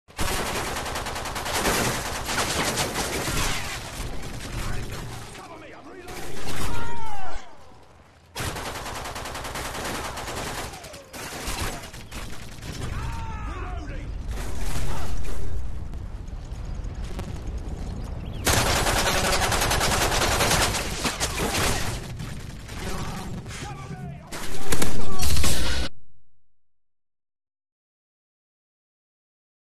FMG9COPTER
Best way to distract enemy then pwn'd out with a main weapon...the saddest way possible. You've been FMG9Copter'd.